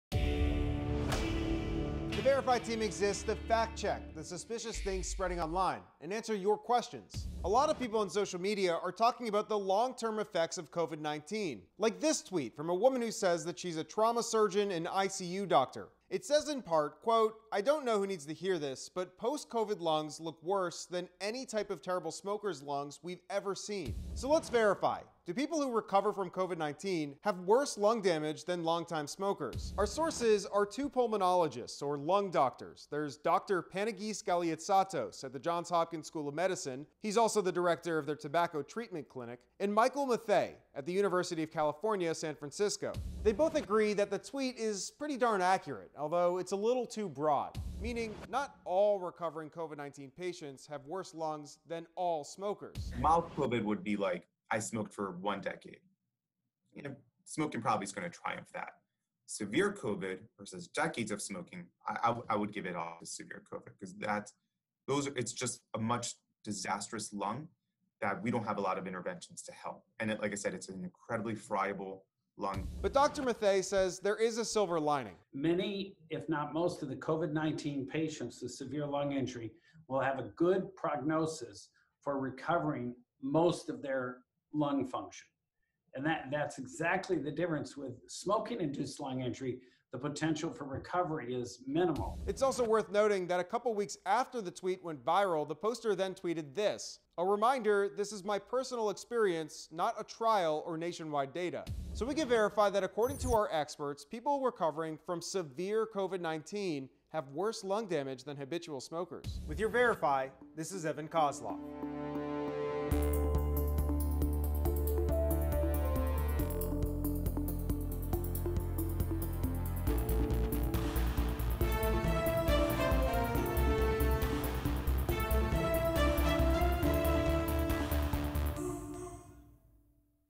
VERIFY: Are post COVID-19 lungs worse than long-time smokers' lungs?
Are post-COVID-19 lungs worse than long-time smokers' lungs?

ANSWER:
It depends on the severity of the COVID-19 case. However, our experts say that it's generally true.
SOURCES:
Dr. Panagis Galiatstatos- Pulmonary and critical care medicine physician, assistant professor in the Division of Pulmonary and Critical Medicine at the Johns Hopkins School of Medicine, director of the Tobacco Treatment Clinic
Dr. Michael Matthay- Professor of Medicine at the University of California, San Francisco
PROCESS:
Lots of people on social media are talking about the long-term effects of COVID-19. One tweet from a woman who says she's a trauma surgeon and ICU doctor, got a lot of buzz online.

"I don't know who needs to hear this, but 'post-Covid' lungs look worse than any type of terrible smoker's lungs we've seen," she wrote. "And they collapse. And they clot off. And the shortness of breath lingers on... & on... & on."

Lots of people began comparing the two online. So we're Verifying:
Do people who recover from COVID have worse lung damage than long-time smokers?

Our Verify researchers spoke with two pulmonologists, doctors who specialize in the respiratory system: Dr. Panagis Galiatsatos at Johns Hopkins School of Medicine and Dr. Michael Matthay, a professor of Medicine at the University of California, San Francisco.

They both agree the tweet is pretty accurate, but it’s a bit too broad -- not all recovering COVID-19 patients have worse lungs than all smokers.

"The challenge we have is it's going to be grey," Galiatsatos said. "Severe COVID-lung would be worse than a patient who smokes's lungs. Mild cases, likely no."

"Mild COVID would be like I smoked for one decade," Galiatsatos later added.

Our Verify researchers asked Dr. Matthay if he thought that someone who sick with COVID inherently has worse lungs than someone who's been smoking for a decade.

"Yes, yes they do," Matthay said. "They definitely do, because the patients who have COVID-19 lung injury and pneumonia, particularly in both lungs, yes, they definitely have worse lungs radiographically and functionally than the patient has just been cigarette smoking for 10 or 20 years."

Dr. Galiatsatos shared x-ray images of patients with the Verify team to show the differences between normal lungs, lungs belonging to a COVID-19 patient and lungs from a long-time smoker:
Healthy lungs (left) are uniform with a little bit of gray to this, showing the gas exchange transpiring.
Lungs of a COVID-19 patient (middle) are more opaque and white, which shows inflammation and an active infection.
Smokers' lungs (right) may have a portion that's dark black which can indicate emphysema.
Both Galiatsatos and Matthay noted that the rate at which your a patients' lungs become damaged by COVID is much faster than by smoking.

"To me, this is kind of like comparing what's worse...a category five hurricane or an earthquake in a large metropolitan area?" Galiatsatos said. "They're both bad. They're just different types of that."

The difference is the rate at which they ravage your organ and induce symptoms like shortness of breath and coughing: one occurs gradually, the other rapidly.

"The severe COVID-19 patient gets to be expedited to that symptom level in a very short period of time," Galiatsatos said. "So both lung imaging are bad, they are horrible, and they'll give you those same symptoms just one much faster than the other one. So to me, I would say someone with severe COVID-19 from SARS-CoV-2 you're experiencing what a person whose smoked extensively for years, they're experiencing those symptoms in an immediate sense, versus having to wait decades."

But Dr. Matthay says there’s a silver lining.
"Many if not most of the COVID-19 patients with severe lung injury will have a good prognosis for recovering most of their lung function, and that that's exactly the difference with smoking-induced lung injury, the potential for recovery is minimal," Matthay said.  He said that data from the past year suggests that those with COVID-19 related lung injuries, should gradually recover.

So we can Verify, according to our experts, yes, in general people recovering from severe COVID-19 have worse lung damage than habitual smokers.
Remember patient recovery is a whole spectrum.